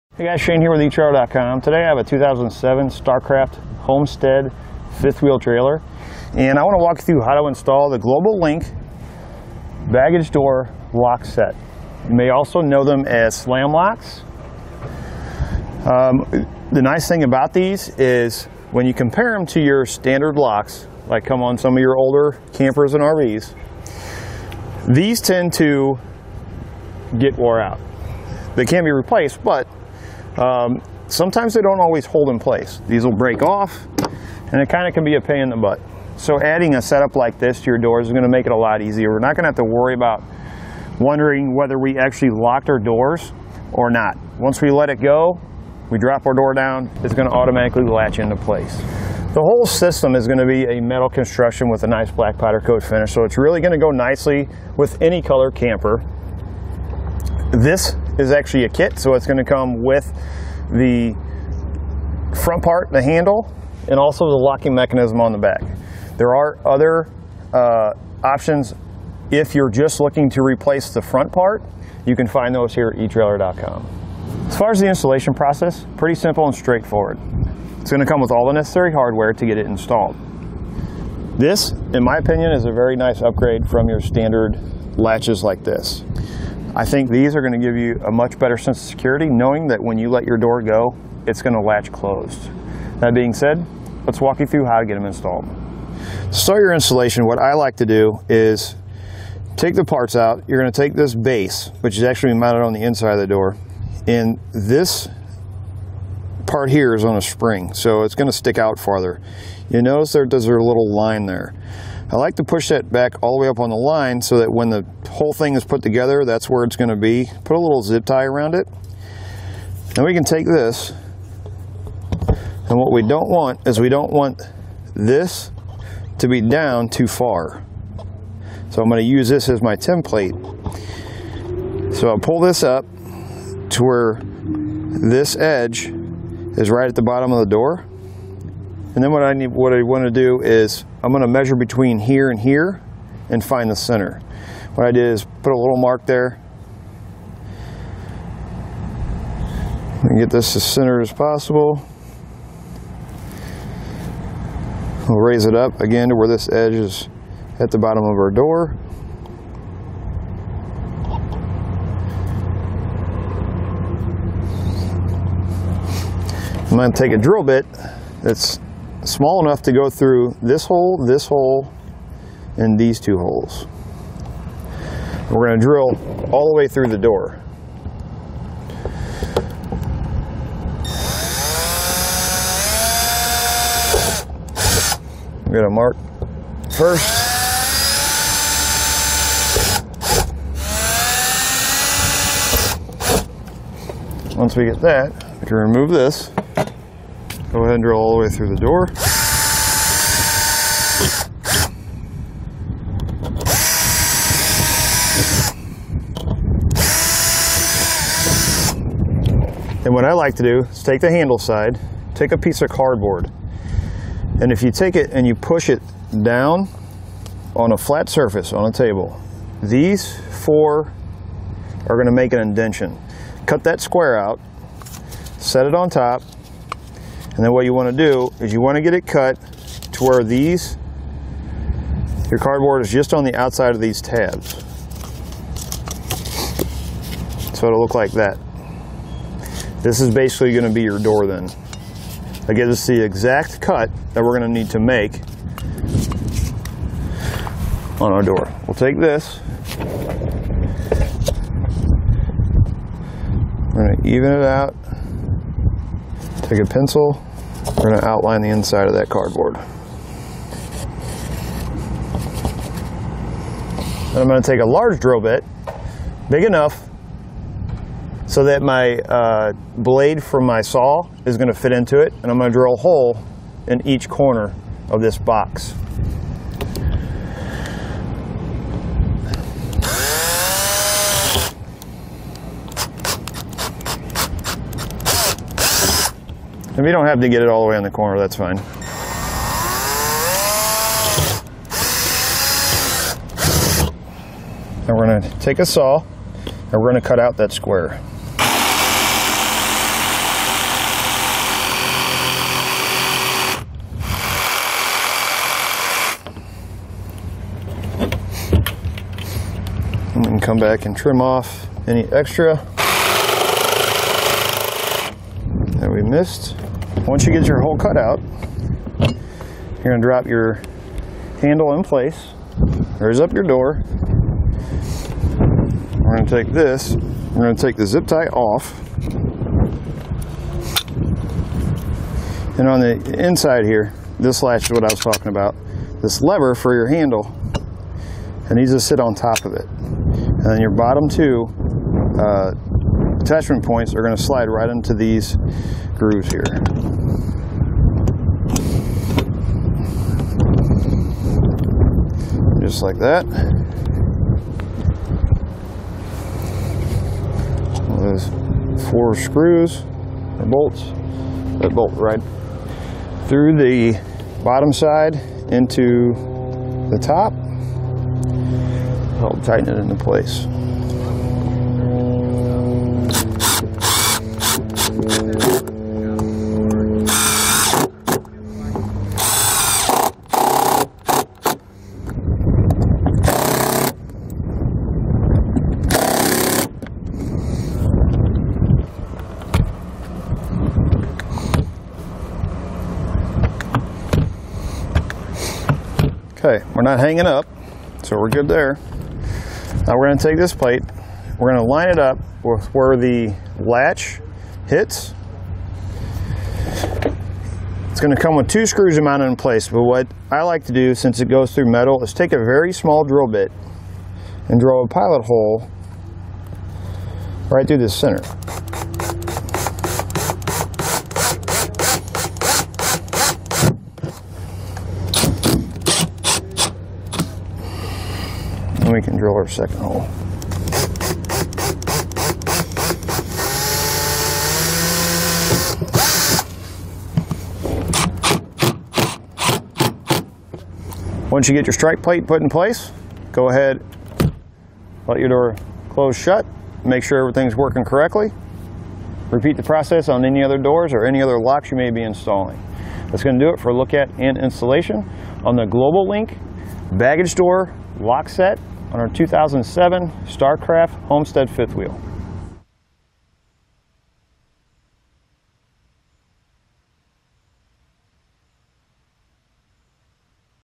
Global Link Locking Baggage Lock Actuator Set Installation - 2007 Starcraft Homestead Lite Fifth Whe
Click for more info and reviews of this Global Link RV Door Parts:

Shane:                                                 Hey guys, Shane here with eTrailer.com. Today I have a 2007 Starcraft Homestead fifth wheel trailer and I want to walk you through how to install the Global Link Baggage Door Lock Set. You may also know them as slam locks.The nice thing about these is when you compare them to your standard locks, that come on some of your older campers and RVs, these tend to get wore out. They can be replaced, but sometimes they don't always hold in place. These will break off and it kind of can be a pain in the butt. So adding a setup like this to your doors is going to make it a lot easier.

We're not going to have to worry about wondering whether we actually locked our doors or not. Once we let it go, we drop our door down, it's going to automatically latch into place.The whole system is going to be a metal construction with a nice black powder coat finish, so it's really going to go nicely with any color camper. This is actually a kit, so it's going to come with the front part, the handle, and also the locking mechanism on the back. There are other options if you're just looking to replace the front part, you can find those here at eTrailer.com.As far as the installation process, pretty simple and straightforward. It's going to come with all the necessary hardware to get it installed.

This, in my opinion, is a very nice upgrade from your standard latches like this. I think these are going to give you a much better sense of security, knowing that when you let your door go it's going to latch closed. That being said, let's walk you through how to get them installed.To start your installation what I like to do is take the parts out. You're going to take this base, which is actually going to be mounted on the inside of the door, and this part here is on a spring so it's going to stick out farther. You notice there there's a little line there.

I like to push that back all the way up on the line, so that when the whole thing is put together that's where it's going to be. Put a little zip tie around it.Then we can take this, and what we don't want is we don't want this to be down too far, so I'm going to use this as my template. So I'll pull this up to where this edge is right at the bottom of the door. Then what I want to do is I'm going to measure between here and here, and find the center. What I did is put a little mark there.

I'm going to get this as centered as possible. I'm going to raise it up, again to where this edge is at the bottom of our door. I'm going to take a drill bit that's small enough to go through this hole, this hole, and these two holes. We're going to drill all the way through the door. We've got to mark first. Once we get that we can remove this. Go ahead and drill all the way through the door.And what I like to do is take the handle side, take a piece of cardboard. If you take it and you push it down on a flat surface, on a table, these four are going to make an indention. Cut that square out, set it on top, and then what you want to do is you want to get it cut to where your cardboard is just on the outside of these tabs. So it'll look like that. This is basically going to be your door then. Again, this is the exact cut that we're going to need to make on our door. We'll take this, we're going to even it out. Take a pencil, we're going to outline the inside of that cardboard.And I'm going to take a large drill bit, big enough so that my blade for my saw is going to fit into it, and I'm going to drill a hole in each corner of this box. If don't happen to get it all the way in the corner, that's fine. We're going to take a saw and we're going to cut out that square. You can come back and trim off any extra that we missed.Once you get your hole cut out you're going to drop your handle in place, raise up your door. We're going to take this, we're going to take the zip tie off. And on the inside here this latch, what I was talking about, this lever for your handle, it needs to sit on top of it. Then your bottom two attachment points are going to slide right into these grooves here. Just like that. There's four screws and bolts that bolt right through the bottom side into the top to help tighten it into place.Okay, we're not hanging up, so we're good there. Now we're going to take this plate,